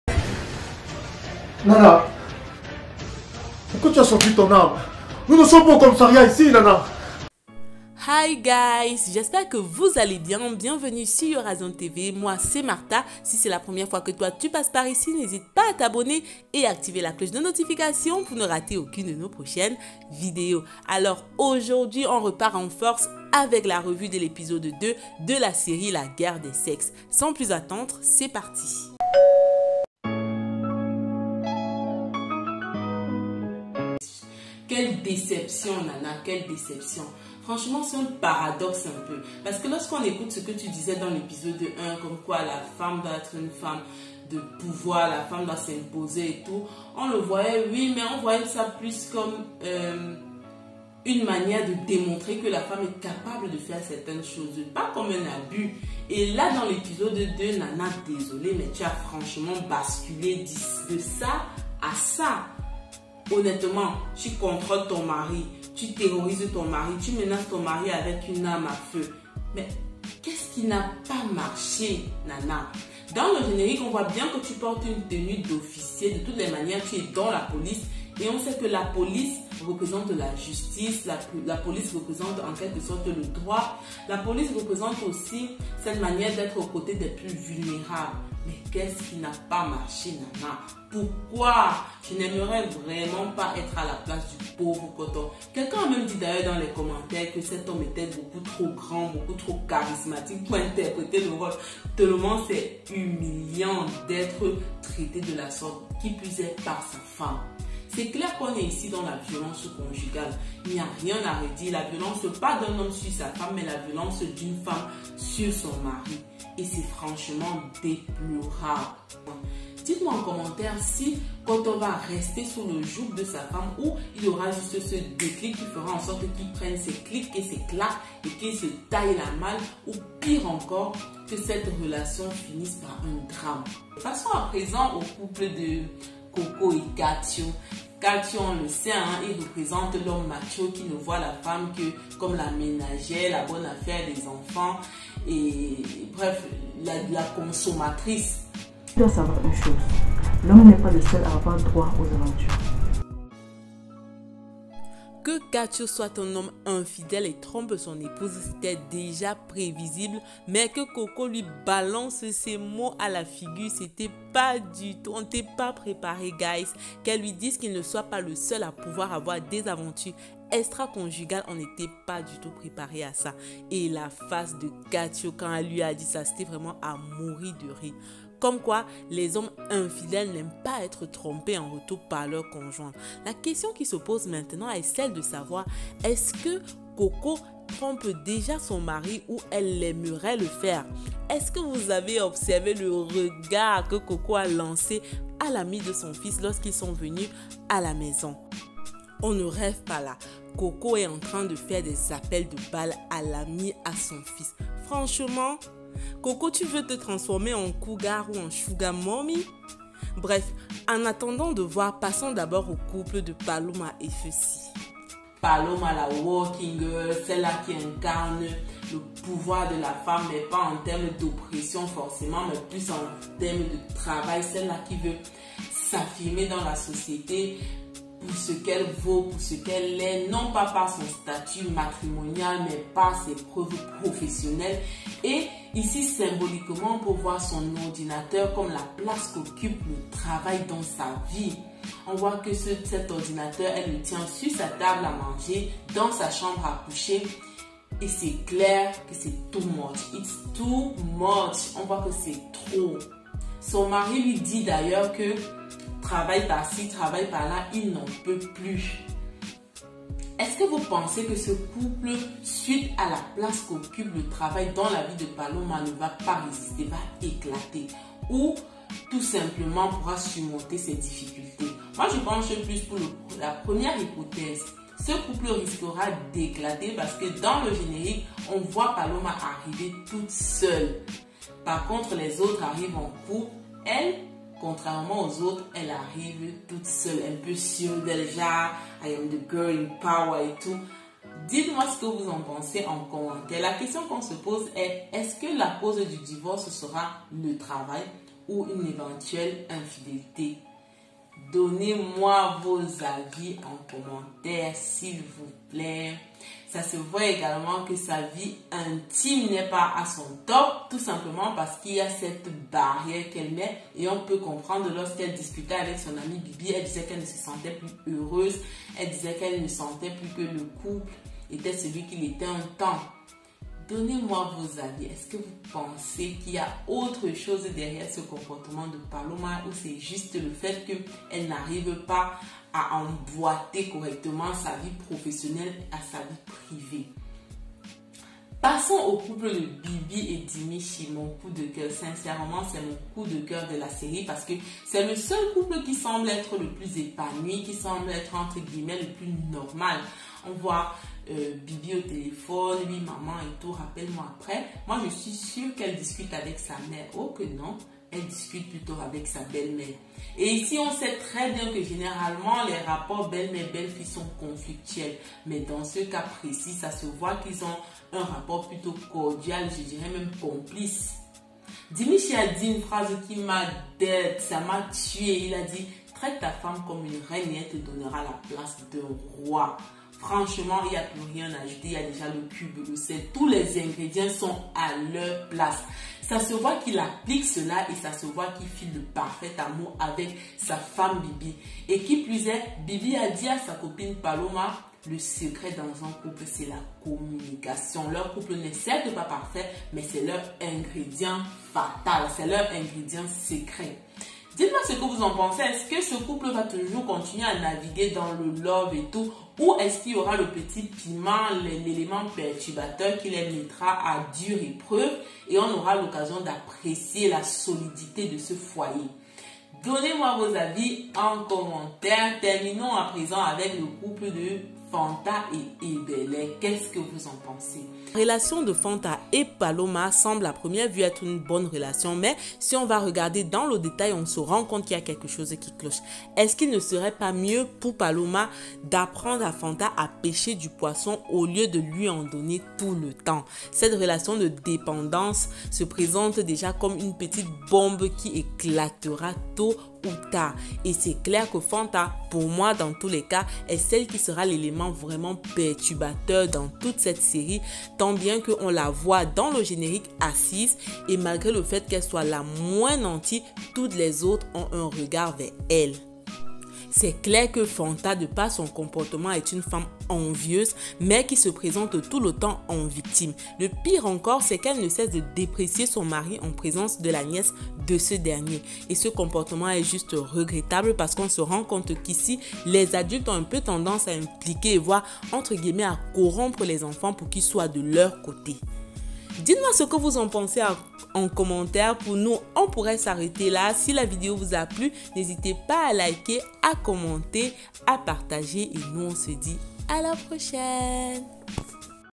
LA GUERRE DES SEXES: ANALYSE EP2
Cette vidéo est une analyse de l'épisode 2 de la série Camerounaise la Guerre des sexes.
si tu aimes la vidéo n'oublie pas de me laisser un 👍 et de partager pour m'encourager.
source:
La guerre des sexes